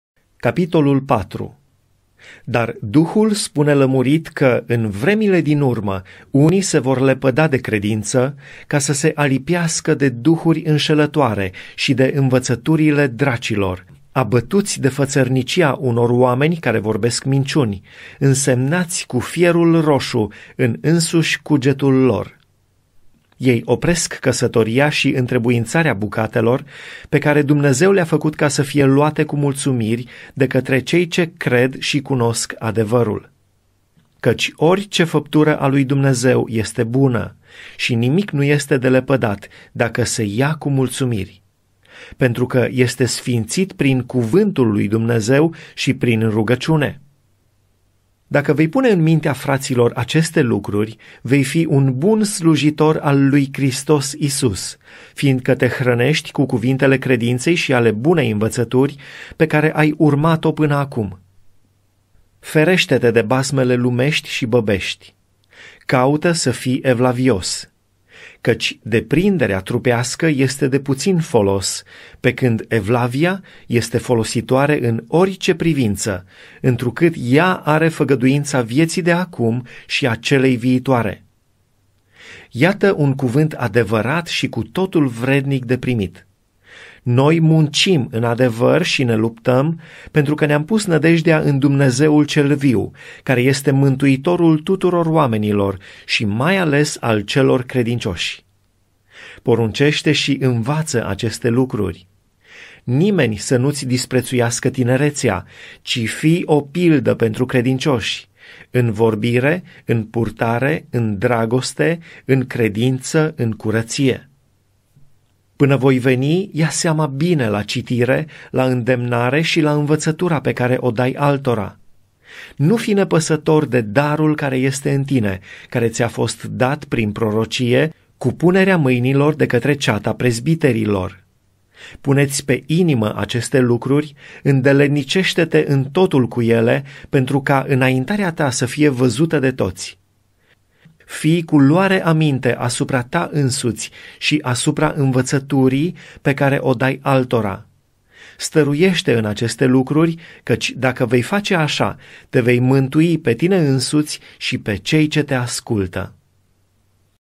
1 TIMOTEI 4
1. Dar Duhul spune lămurit că, în vremurile din urmă, unii se vor lepăda de credinţă, ca să se alipească de duhuri înşelătoare şi de învăţăturile dracilor,
2. abătuţi de făţărnicia unor oameni care vorbesc minciuni, însemnaţi cu fierul roşu în însuşi cugetul lor.